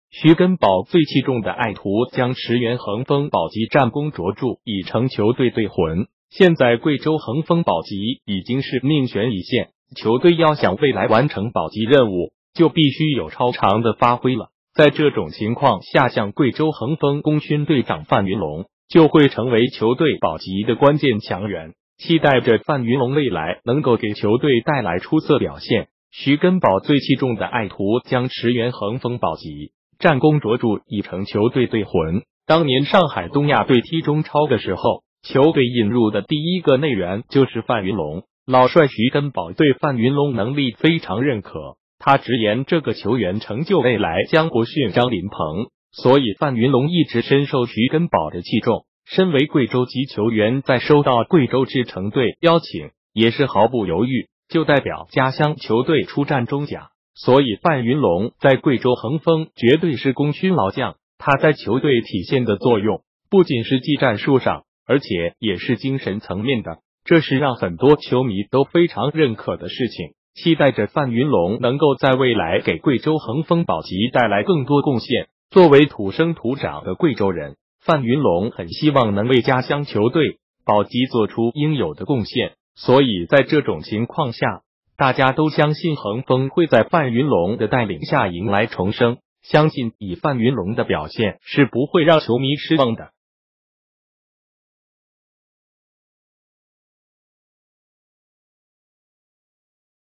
徐根寶最器重的愛徒將馳援恒豐保級 戰功卓著已成球隊隊魂！
現在貴州恒豐保級已經是命懸一線，球隊要想未來完成保級任務就必須有超常的發揮了。在這種情況下像貴州恒豐功勛隊長范云龍，就會成為球隊保級的關鍵強援。